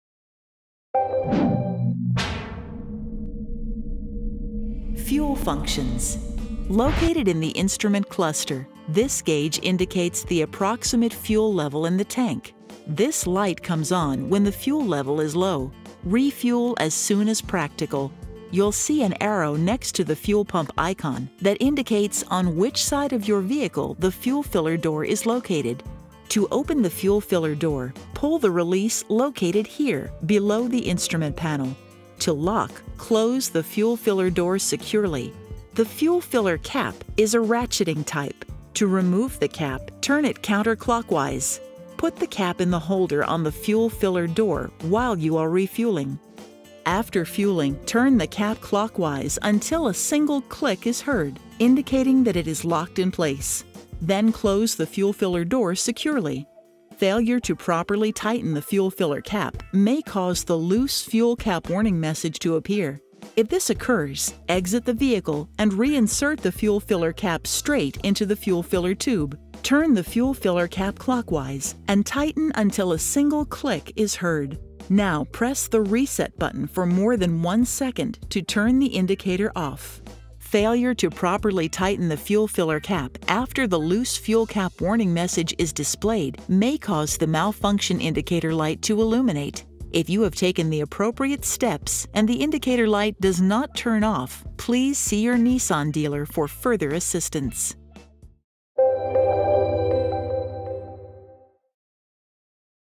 2013 NISSAN Juke - Fuel Functions
Fuel Functions gas cap fuel filler door loose fuel cap cars 2013 Nissan Juke EQRG Electronic Quick Reference Guide  how to "Fuel Functions

Located in the instrument cluster, this gauge indicates the approximate fuel level in the tank. This light comes on when the fuel level is low. Refuel as soon as practical. You'll see an arrow next to the fuel pump icon that indicates on which side of your vehicle the fuel-filler door is located. To open the fuel-filler door, pull the release located here, below the instrument panel. To lock, close the fuel-filler door securely. The fuel-filler cap is a ratcheting type. To remove the cap, turn it counterclockwise. Put the cap in the holder on the fuel-filler door, while you are refueling.

After fueling, turn the cap clockwise until a single click is heard, indicating that it is locked in place. Then close the fuel-filler door securely. Failure to properly tighten the fuel-filler cap may cause the Loose Fuel Cap warning message to appear. If this occurs:
1. Exit the vehicle and reinsert the fuel-filler cap straight into the fuel-filler tube.
2. Turn the fuel-filler cap clockwise and tighten until a single click is heard.
3. Now, press the RESET button for more than one second to turn the indicator OFF. Failure to properly tighten the fuel-filler cap after the Loose Fuel Cap warning message is displayed may cause the Malfunction Indicator Light to illuminate. If you have taken the appropriate steps and the indicator light does not turn off, please see your NISSAN dealer for further assistance."